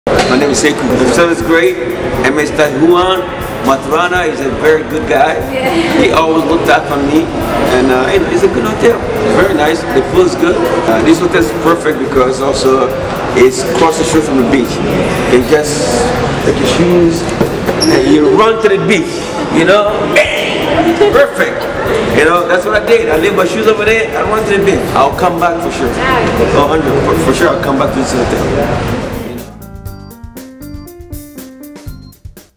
Cartagena Plaza Hotel Review - In Cartagena de Indias Front on the beach
Cartagena Plaza Hotel, member of the Dorado Plaza Hotel Organization started its operations back in the year 1993. The hotel counts with 311 comfortable rooms divided into Standards, Executive, Superior, a Suite and a Junior Suite with the best view towards the sea and the Old city, a great panoramic embracing you to the magic of the Caribbean.

Just 10 minutes far from Rafael Núñez International Airport and five minutes from the Old city, and with a privileged location in front of the sea and a few steps from casinos, recognized brand stores, we are willing to become your best ally in order to achieve all your activities’ success.